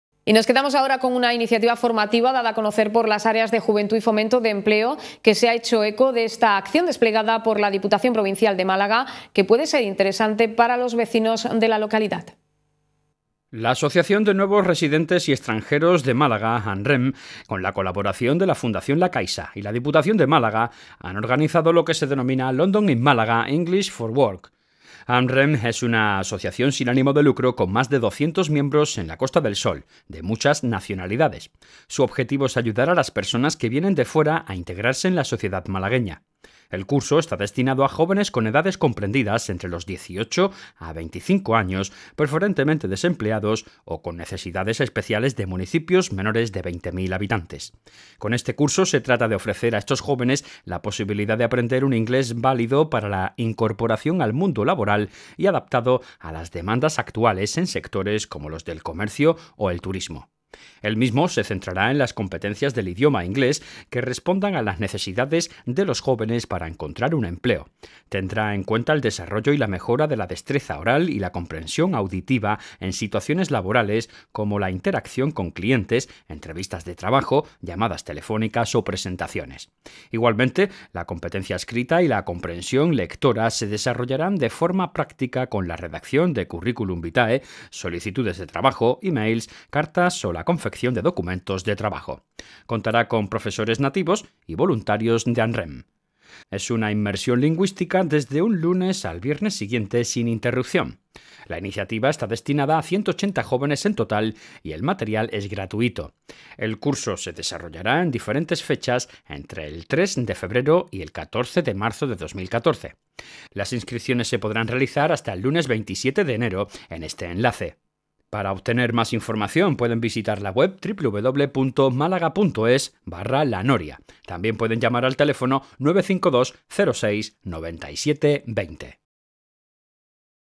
DIPUTACIÓN PONE EN MARCHA CURSOS DE INGLÉS PARA JÓVENES
La Asociación de Nuevos Residentes y Extranjeros de Málaga (ANREM), con la colaboración de la Fundación La Caixa y La Diputación de Málaga (La Noria) han organizado lo que se denomina "LONDON IN MÁLAGA II -- ENGLISH FOR WORK".

ANREM es una asociación sin ánimo de lucro, con más de 200 miembros en la Costa del Sol, de muchas nacionalidades. Su objetivo es ayudar a las personas que vienen de fuera a integrarse en la sociedad malagueña.

El curso esta destinado a jóvenes con edades comprendidas entre los 18 a 25 años, preferentemente desempleados o necesidades especiales de municipios menores de 20.000 habitantes.

Con este curso se trata de ofrecer a estos jóvenes la posibilidad de aprender un inglés válido para la incorporación al mundo laboral y adaptado a las demandas actuales, en sectores como los del comercio o el turismo.

El mismo se centrará en las competencias del idioma inglés que respondan a las necesidades de los jóvenes para encontrar un empleo. Tendrá en cuenta el desarrollo y la mejora de la destreza oral y la comprensión auditiva en situaciones laborales como la interacción con clientes, entrevistas de trabajo, llamadas telefónicas o presentaciones. Igualmente, la competencia escrita y la comprensión lectora se desarrollarán de forma práctica con la redacción de CV, solicitudes de trabajo, emails, cartas o la confección de documentos de trabajo.  Contará con profesores nativos y voluntarios de ANREM.

Es una inmersión lingüística desde un lunes al viernes siguiente, sin interrupción.

La iniciativa esta destinada a  180 jóvenes en total y el material es gratuito.

El curso se desarrollará en diferentes fechas entre el 3 de febrero y el 14 de marzo de 2014.

Las inscripciones se podrán realizar hasta el lunes 27 de enero en el enlace,